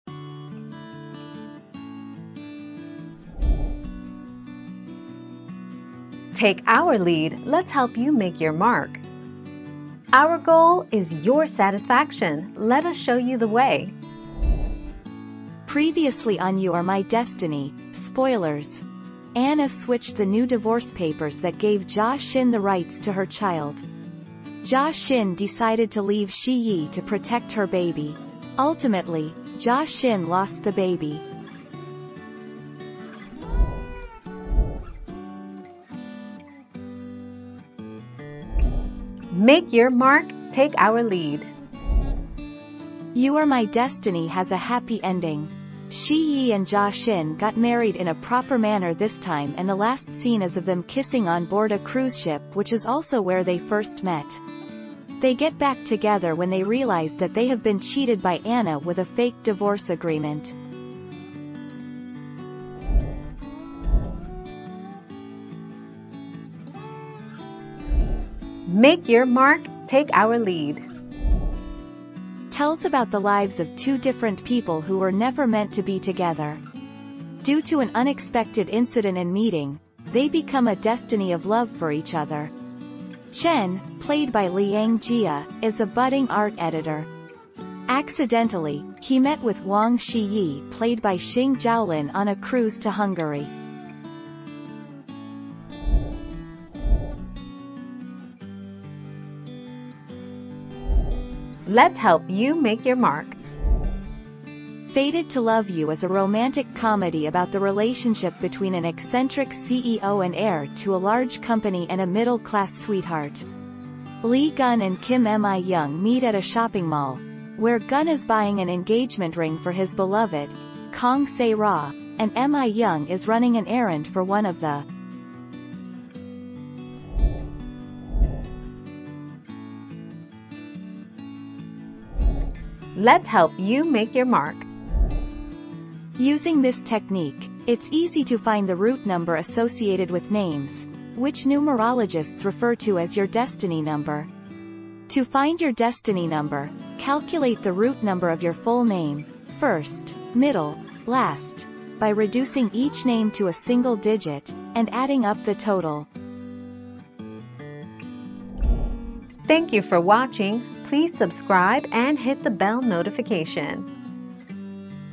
Does the baby die in You Are My Destiny?
00:00 - Does the baby die in You Are My Destiny?
00:35 - How does you're my destiny end?
01:04 - What is the story of you are my destiny?
01:37 - What happens in Fated To Love You?
02:08 - How do I find my destiny?

Laura S. Harris (2021, May 7.) Does the baby die in You Are My Destiny?
    AskAbout.video/articles/Does-the-baby-die-in-You-Are-My-Destiny-242348